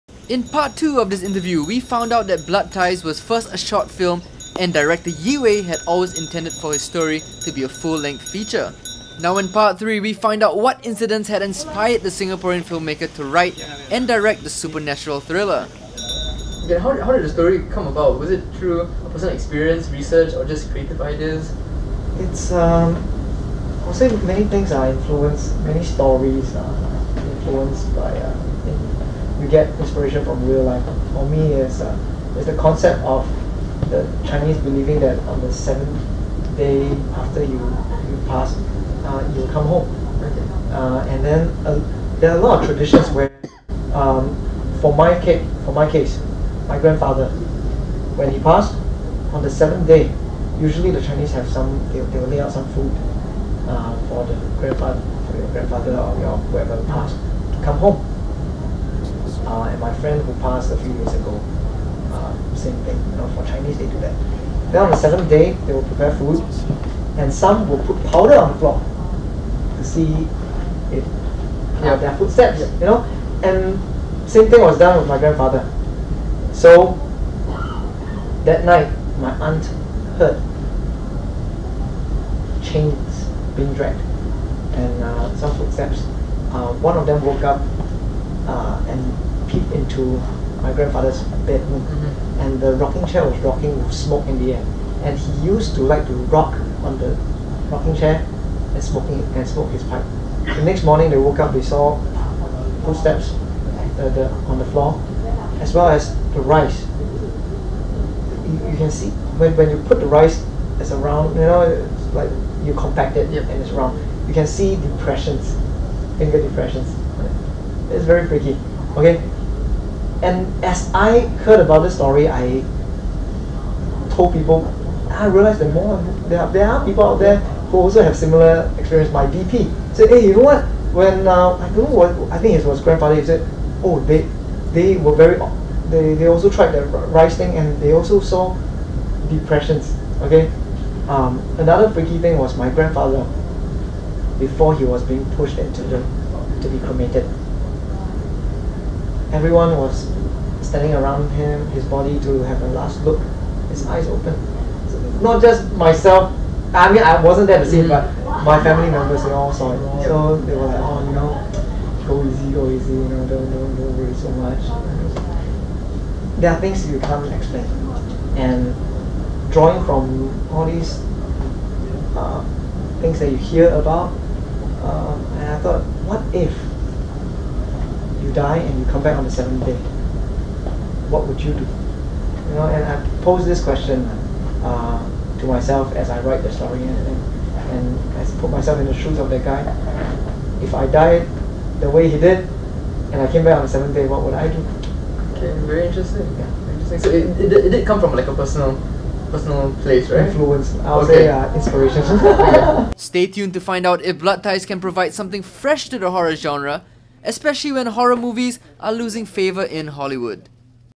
Horror inspired by real life (Blood Ties Pt 3)
In part 2 of this interview, we found out that Blood Ties started as a short film but director Chai Yee-Wei had always intended for it to be a full length feature film. In part 3, we find out what incidents close to home inspired the Singaporean film-maker to write and direct the supernatural thriller.